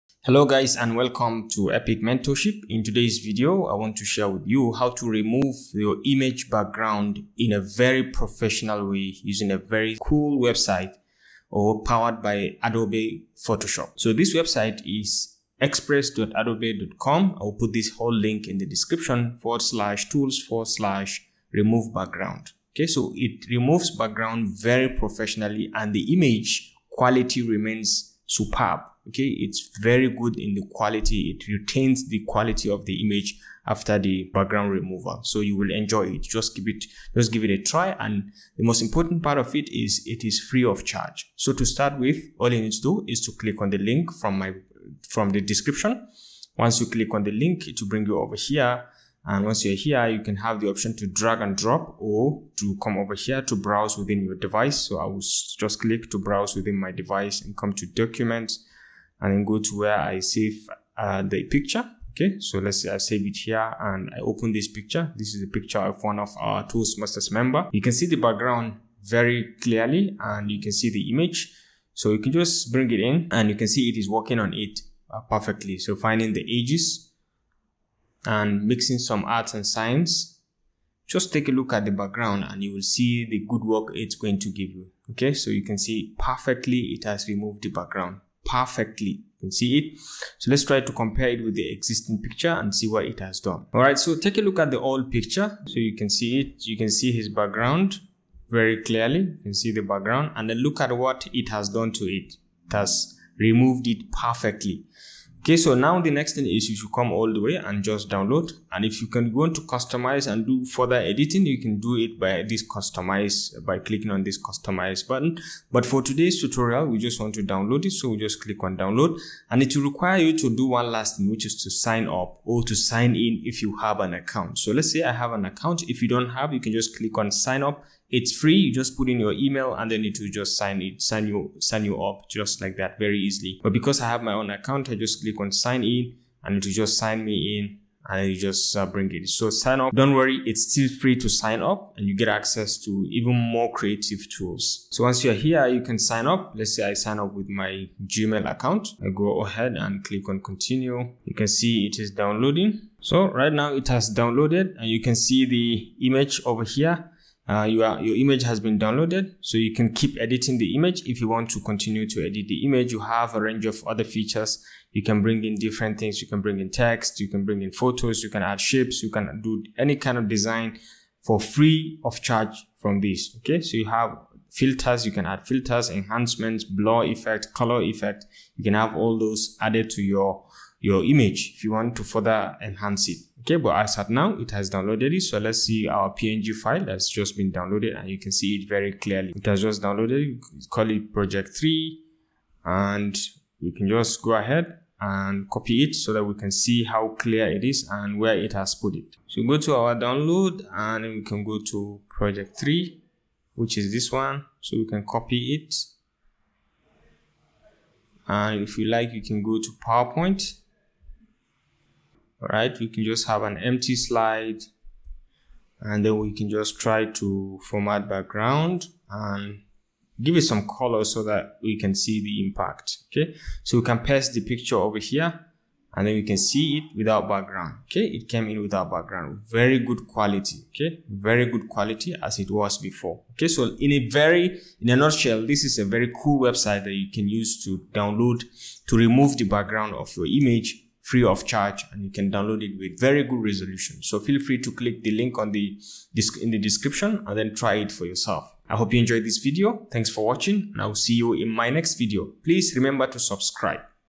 How to Remove Image Background in Adobe Express
In this video, you will learn how to remove image background using adobe express. It is a very easy step-by-step method to show you in just a few clicks how to easily remove image background without using adobe photoshop. Removing background of your image can come handy when you are trying to make the image transparent and use it in your content.

👉Fitbit Versa 3 Smartwatch
👉Anker SoundCore 2 Portable Bluetooth Speaker
👉Nintendo Switch Console
👉GoPro HERO10 Black
👉Roomba i7+ Robot Vacuum